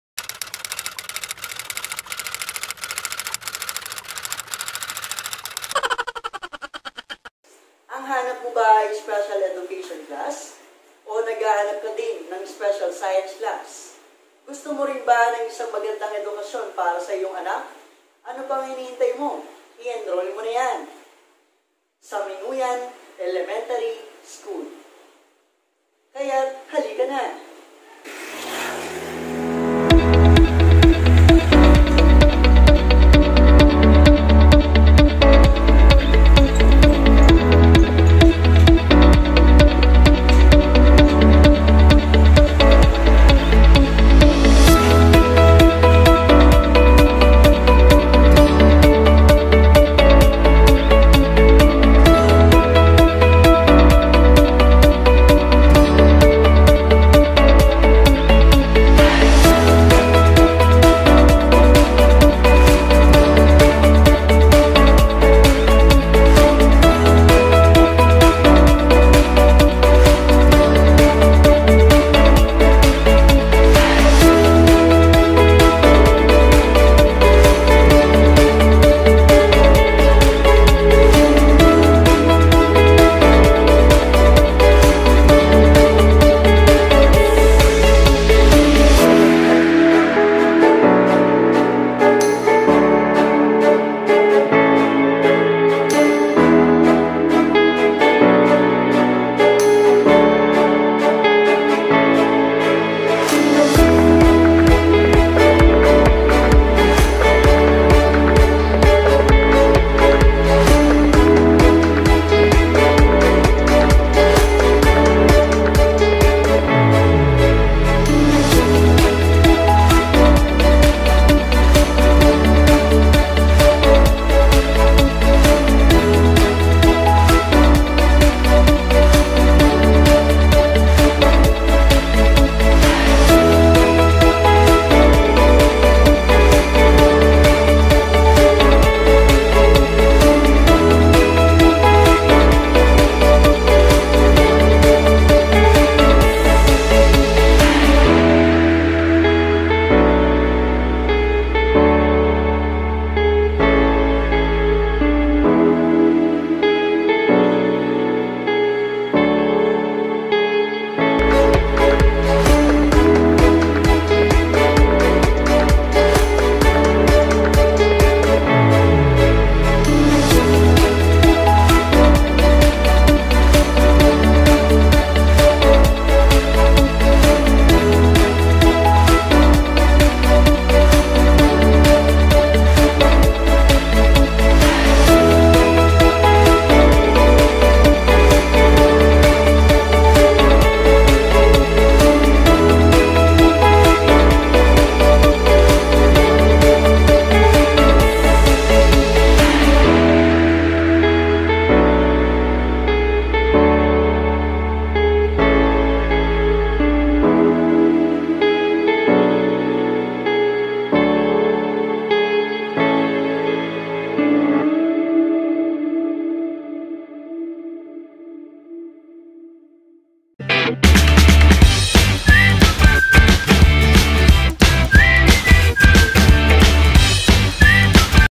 Early Registration Minuyan Elementary School HD 720p
Early Registration.

Enrolment na Naman!...
Kaya ano pa ang hinihintay nyo?
Mag-enrol na sa pinakamalapit na paaralan sa inyong Barangay.. 😁